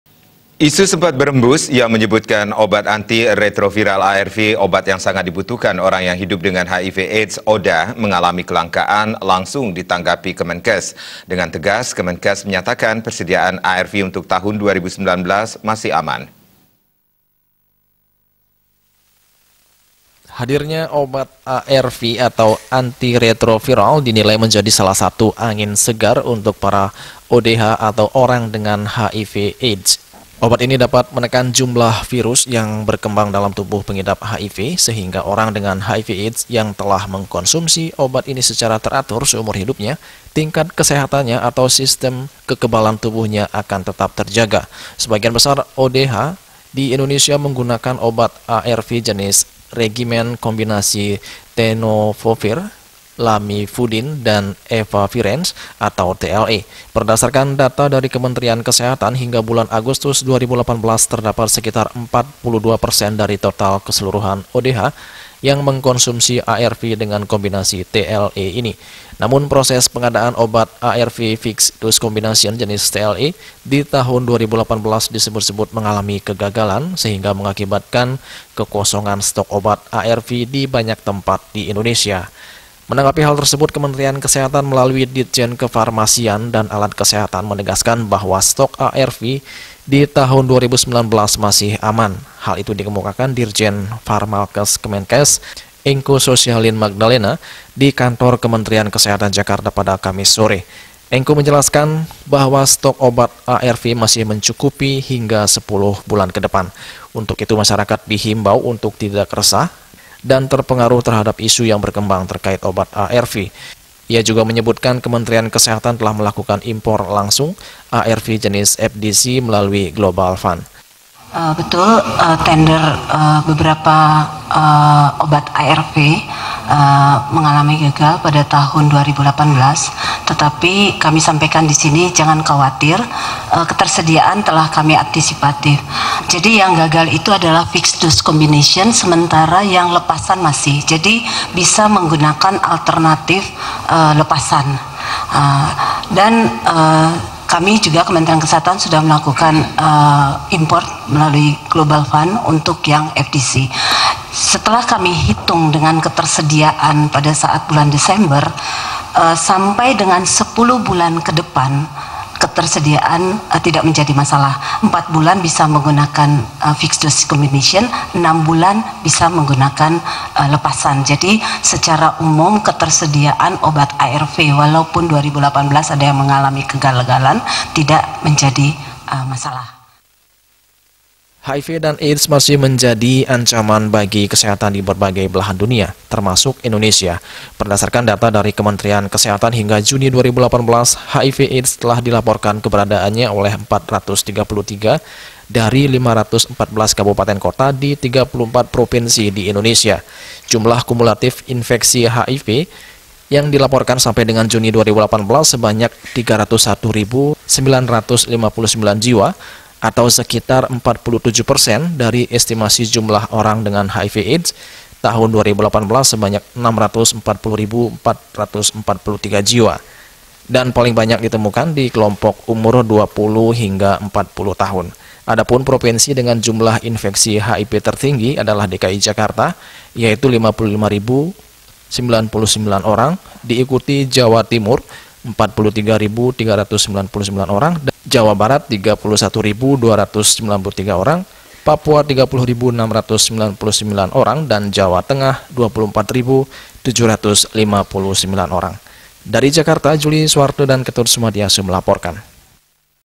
KETERSEDIAAN ARV TAHUN 2019 AMAN
ISU SEMPAT BEREMBUS YANG MENYEBUT OBAT ANTI-RETROVIRAL (ARV) OBAT YANG SANGAT DIBUTUHKAN ORANG YANG HIDUP DENGAN HIV/AIDS, ODHA MENGALAMI KELANGKAAN, LANGSUNG DITANGGAPI KEMENKES. DENGAN TEGAS KEMENKES MENYATAKAN PERSEDIAAN ARV UNTUK TAHUN 2019 MASIH AMAN.